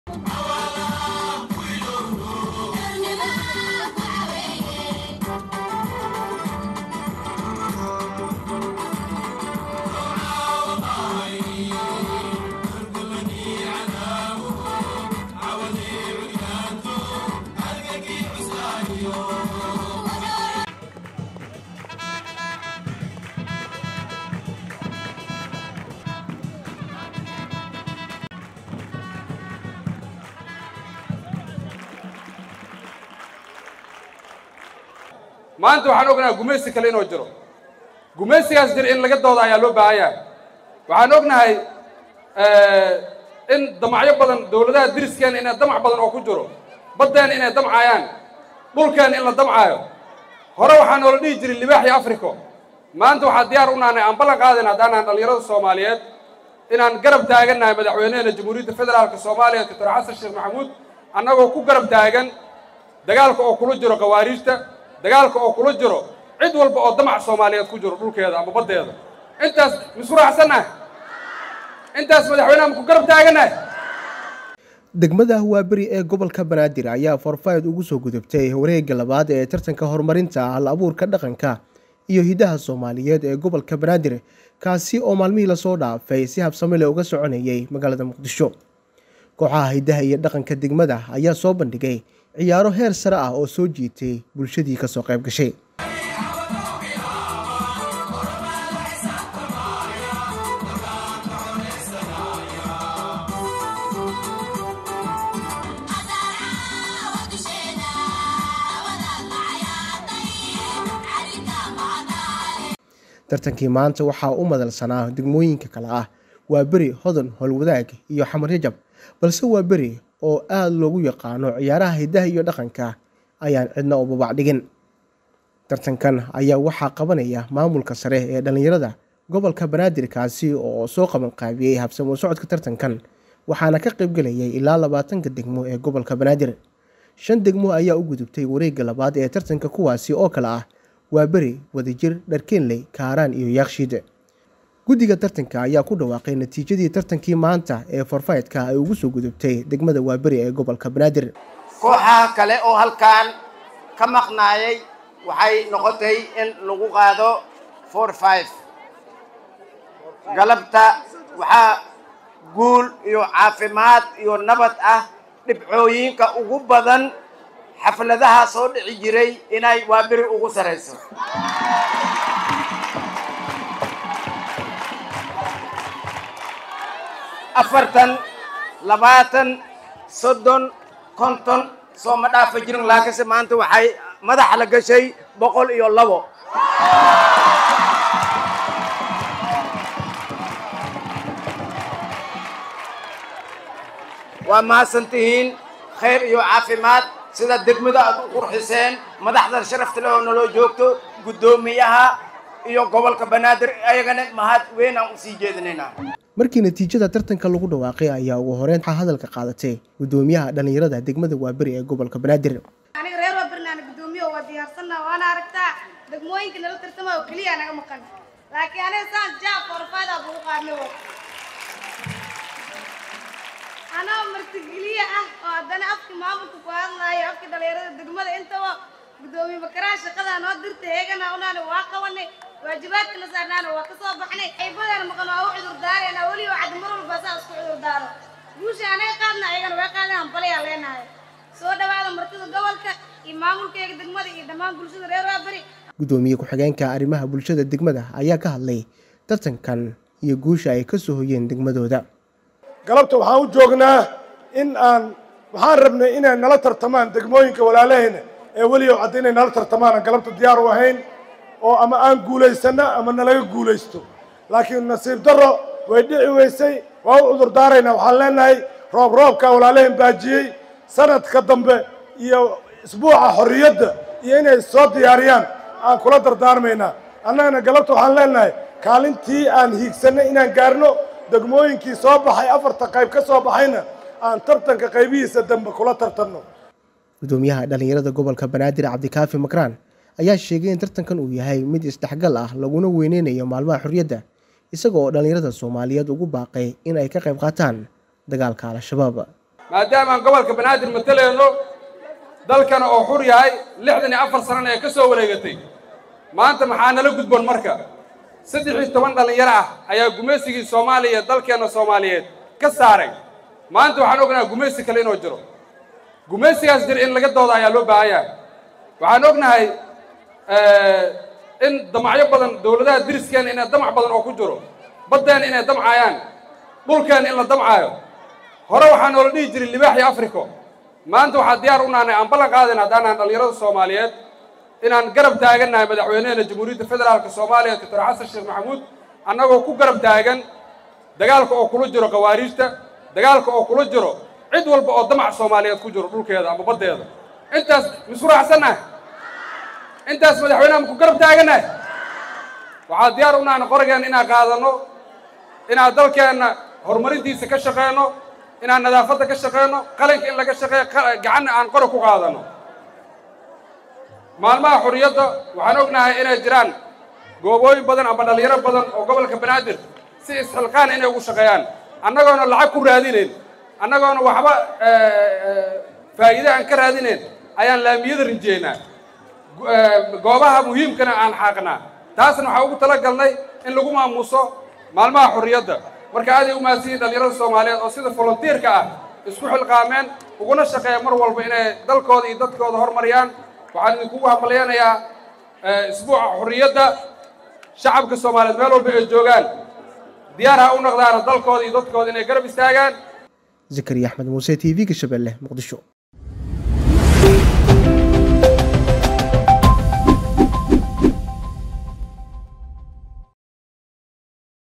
Degmada Waabari oo wacdaro ka dhigtay tartanka hal-abuurka iyo dhaqanka Soomaaliyeed ee.............
Degmada Waabari oo wacdaro ka dhigtay tartanka hal-abuurka iyo dhaqanka Soomaaliyeed ee gobolka Banaadir una gudubtay wareega 2aad ee tartanka.